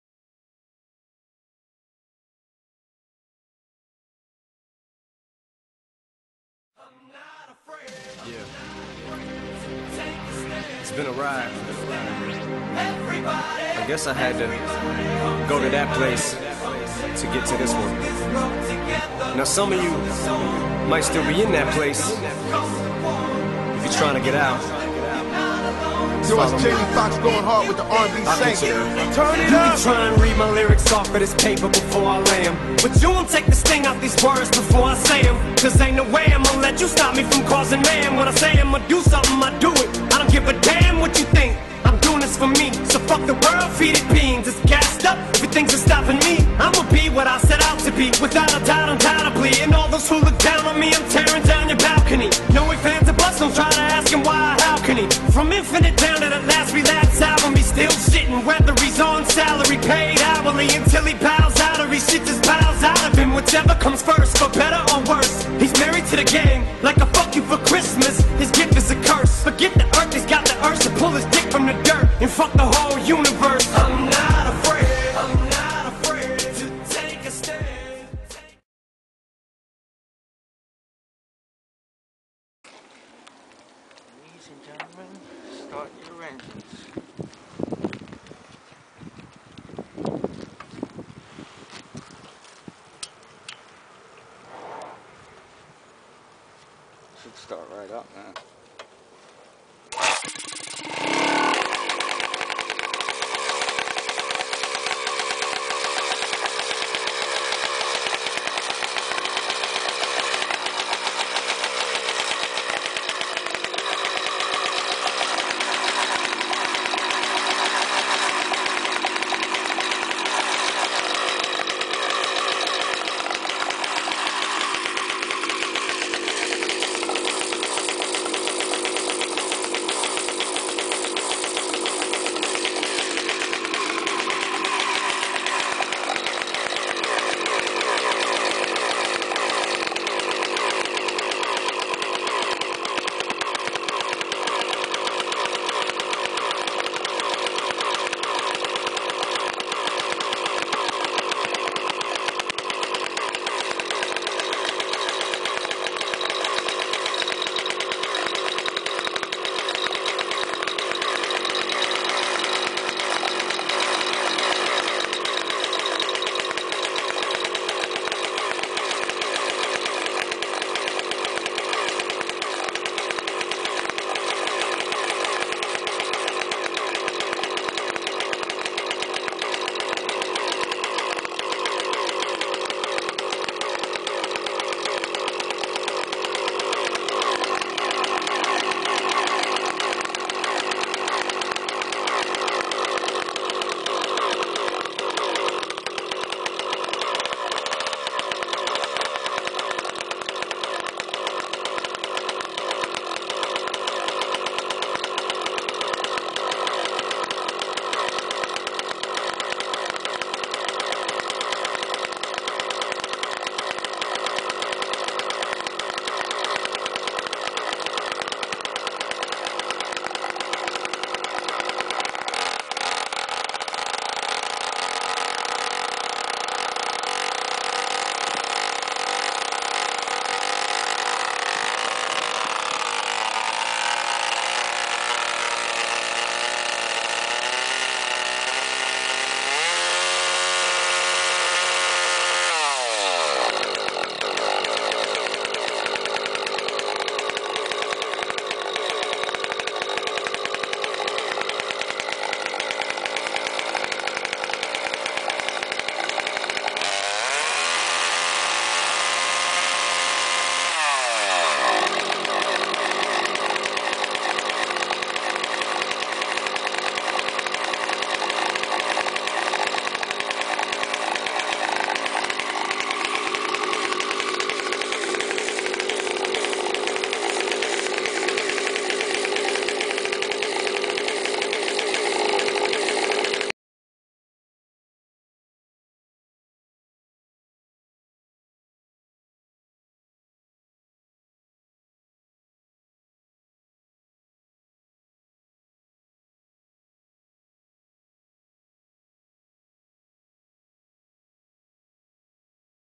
1/4 scale pacesetter dragster build
Here is a short video showing some pictures of my 2011 pacesetter dragster
, all parts are fabricated in house

This car will be used for our 2011 season of 1/4 scale drag racing in Brooklyn NY at Floyd Bennett Field.

Features: 90cc solo motor, New Era quick change rear end.